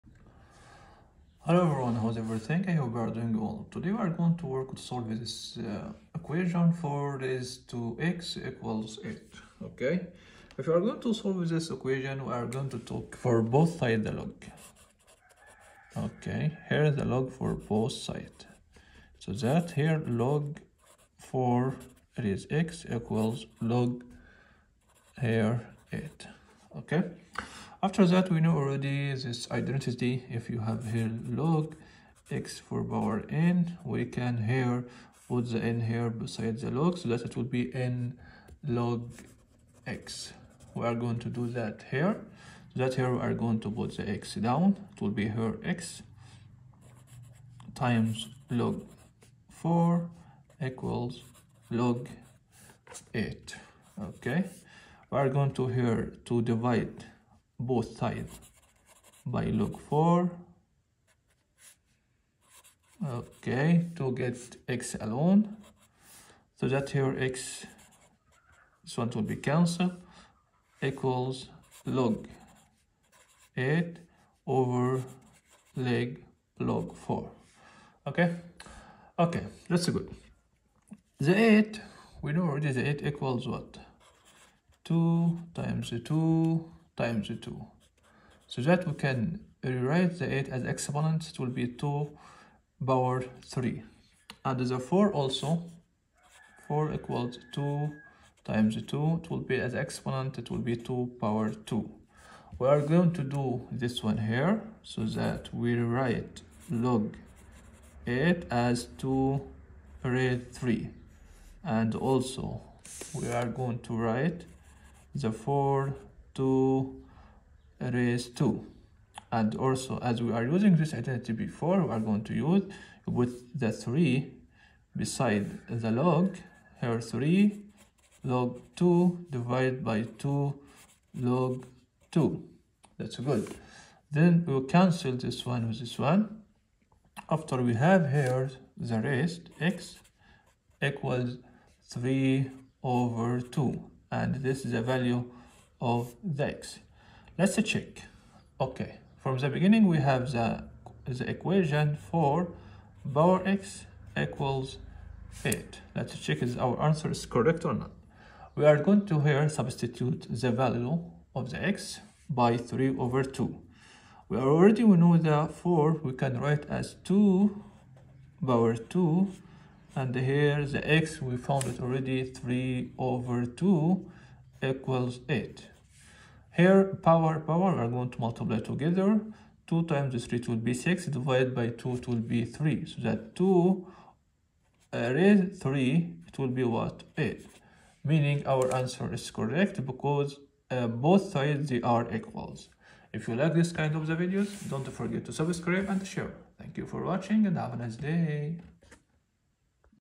Exponentiaol equation المعادلة الاسية
حل المعادلة الاسية باستخدام اللوغارتيمات